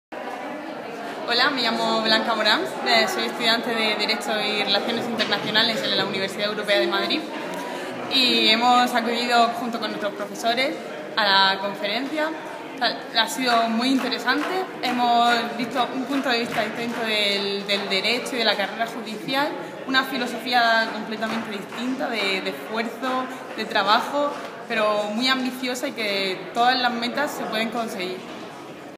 "El equilibrio entre poderes: Treinta años en el Tribunal Supremo de los Estados Unidos"
Blanca Morán, estudiante de Derecho y RRII en la Universidad Europea de Madrid, nos habla sobre el encuentro "El equilibrio entre poderes: Treinta años en el Tribunal Supremo de los Estados Unidos", a cargo de Sandra Day O´Connor, primera Magistrada del Tribunal Supremo de los Estados Unidos, y Antonio Garrigues Walker, Presidente de Garrigues.

Más información en Twitter